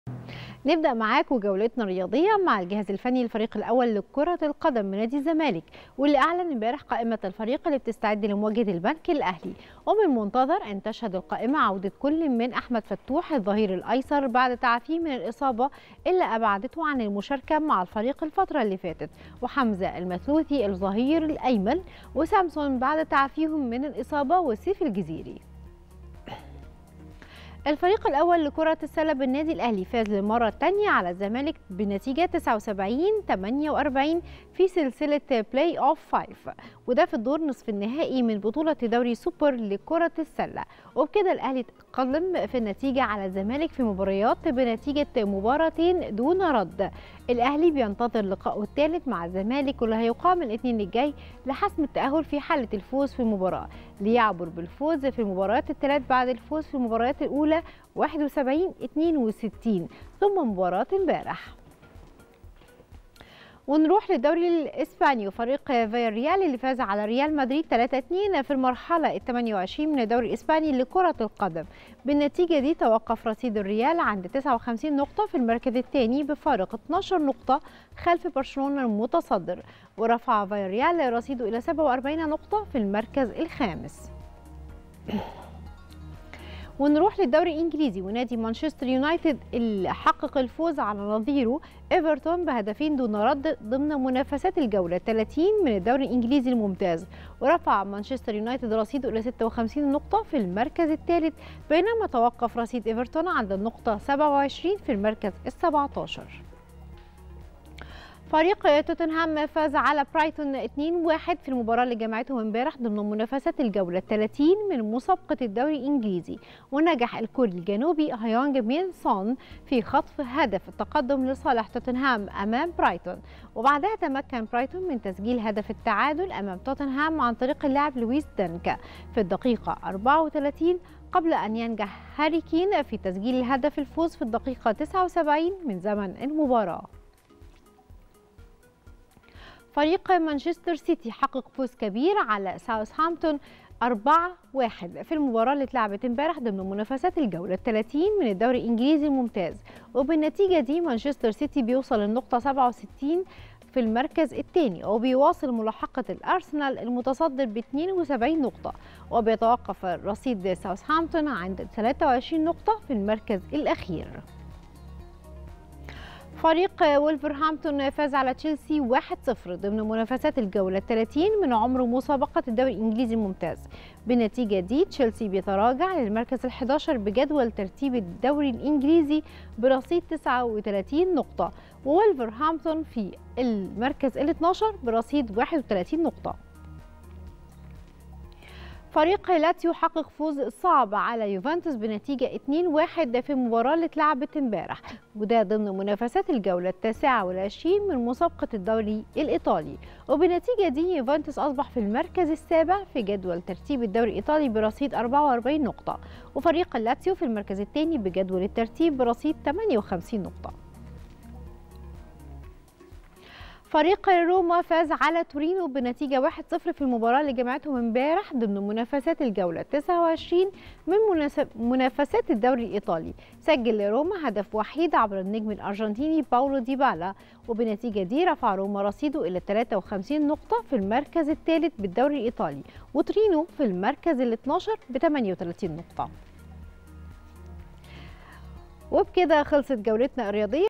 النشرة الرياضية.. عودة فتوح والمثلوثي.. قائمة الزمالك لمواجهة البنك الأهلي في الدوري المصري
صباح الخير يا مصر | النشرة الرياضية.. عودة فتوح والمثلوثي.. قائمة الزمالك لمواجهة البنك الأهلي في الدوري المصري